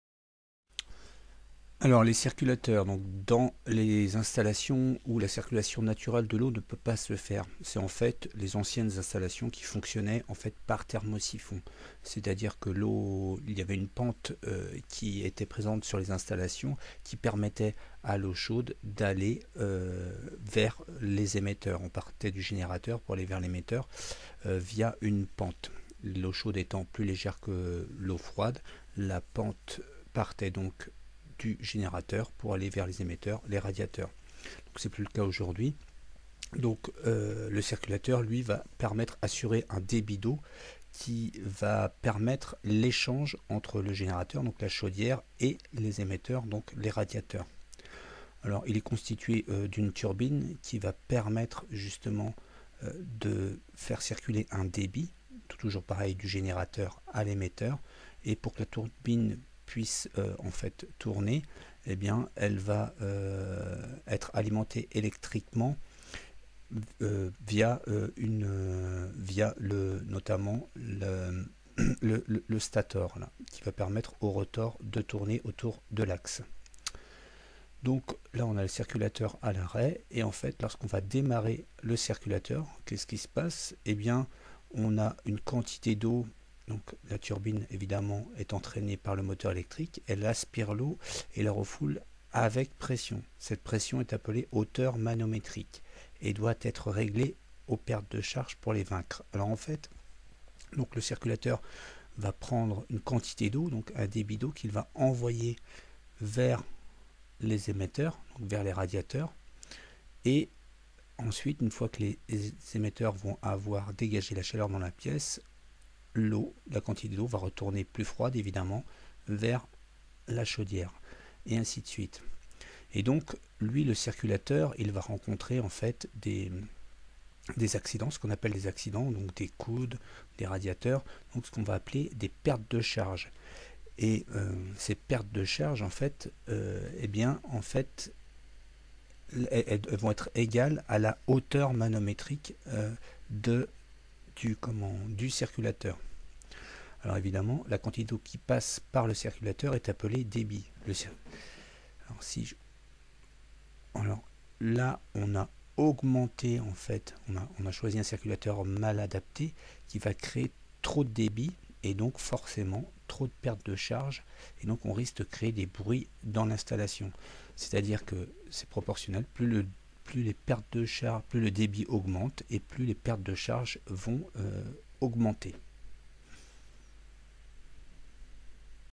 Le circulateur chauffage
Principe de fonctionnement simple du circulateur